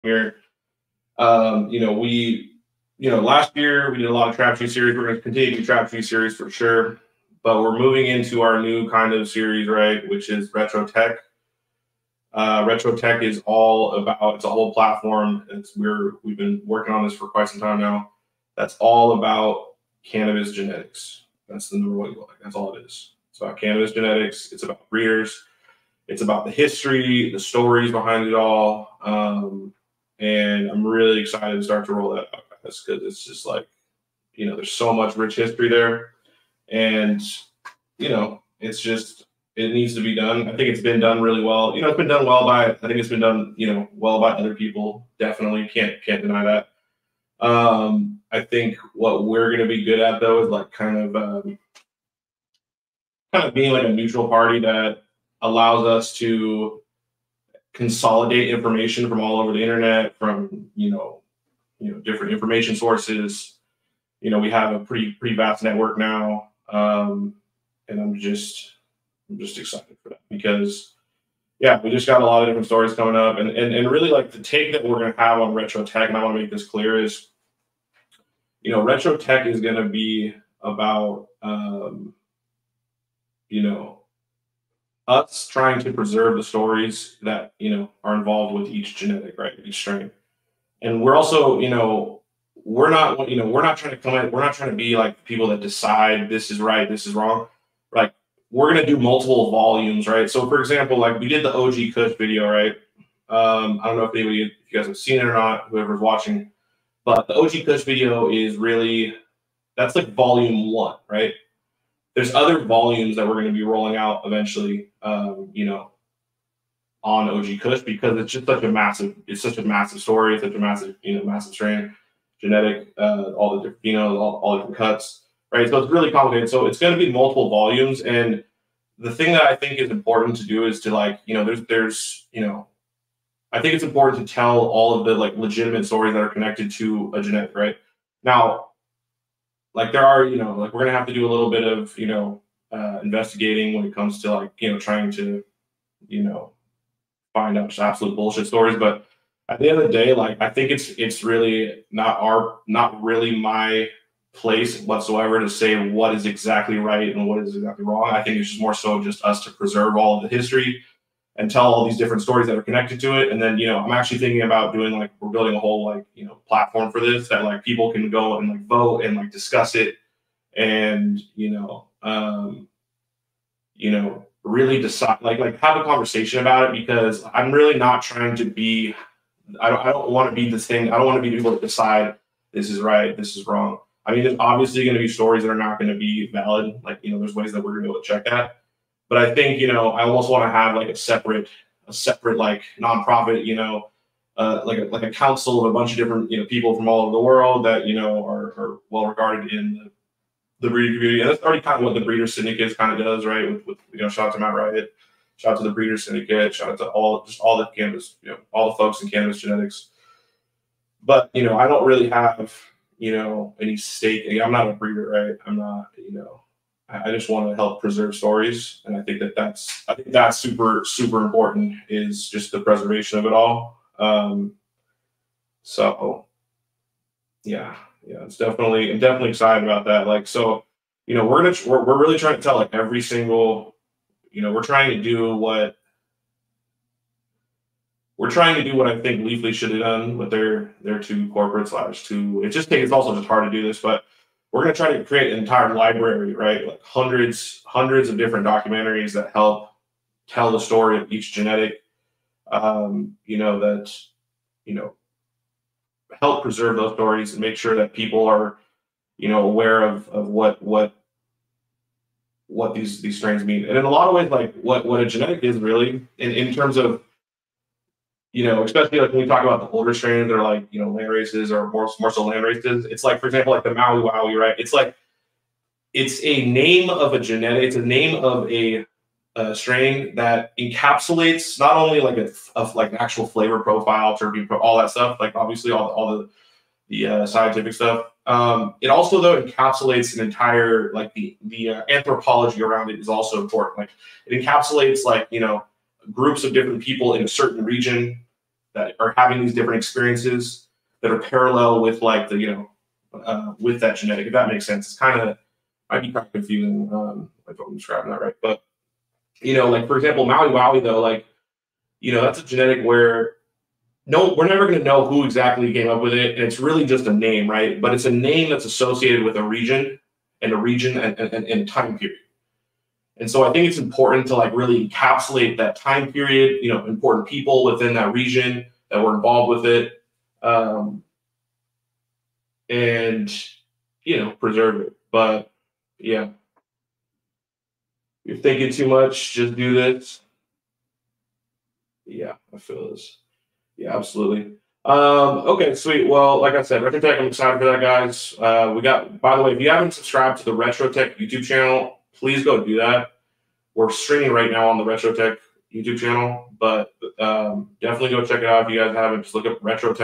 Retro Tech: A Cannabis Strain Story Telling Platform by LMC
Retro Tech: A Cannabis Strain Story Telling Platform by LMC / WHICH CANNABIS BREEDERS SHOULD WE DO A DOCUMENTARY ON FOR RETRO TECH? @RetroTechStories

Welcome to Retro Tech, a unique storytelling platform created by LMC to showcase the rich history and culture behind different cannabis strains. Our marijuana strain history documentaries will take you on a journey through time and space, exploring the origins and evolution of some of the most popular and obscure strains. Join LMC as we delve into the world of cannabis and its fascinating history.

LMC Productions x High Design Media